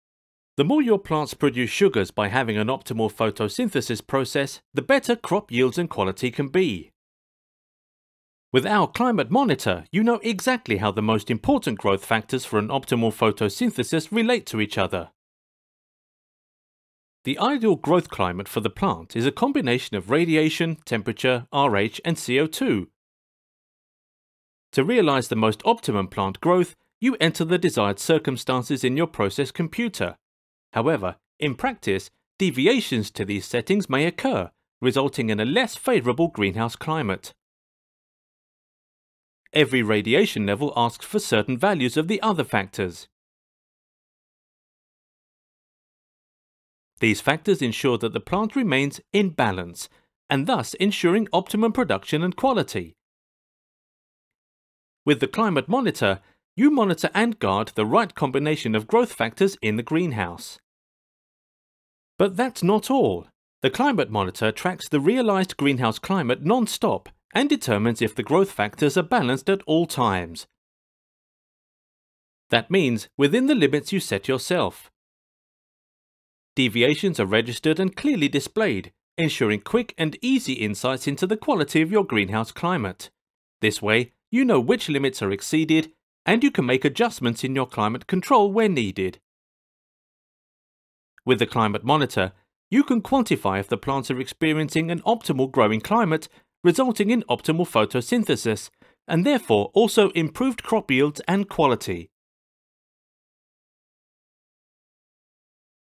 Climate monitor
The better the greenhouse climate, the better crop yields and quality. With the climate monitor you know exactly how the most important growth factors relate to each other.